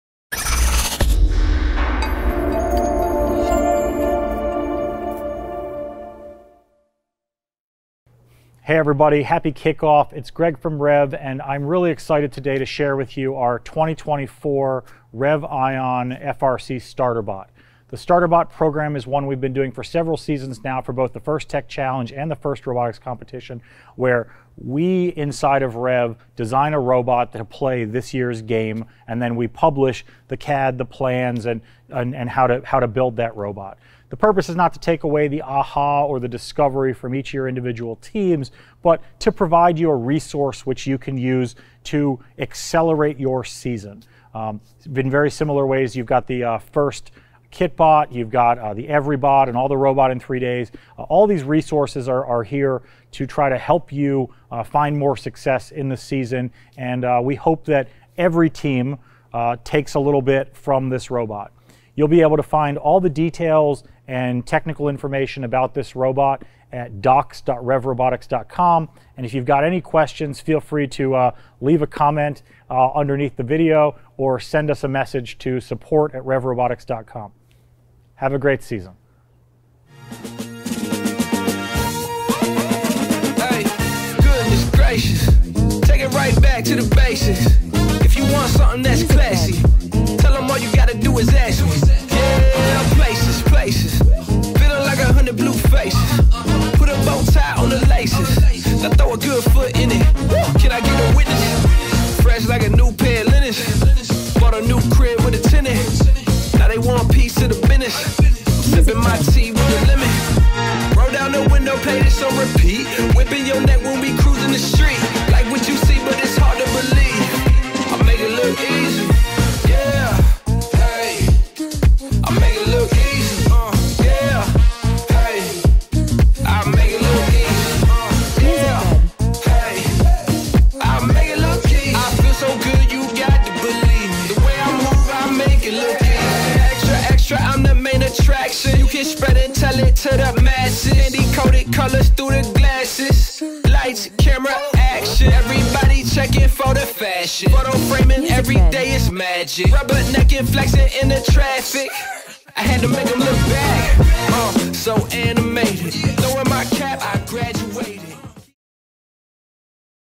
Introducing the 2024 REV ION FRC Starter Bot
Meet the 2024 REV ION FRC Starter Bot! Using all REV ION parts, the Starter Bot is the culmination of analyzing the game, prototyping, and technical documentation. The 2024 REV ION FRC Starter Bot is designed to give teams a starting point for building their robot, while leaving room for iteration, revision, and adaptation to the game’s challenges.

Read more about the 2024 REV ION FRC Starter Bot on our

Visit our technical resource series, FRC Kickoff Concepts for CAD, build guides, and more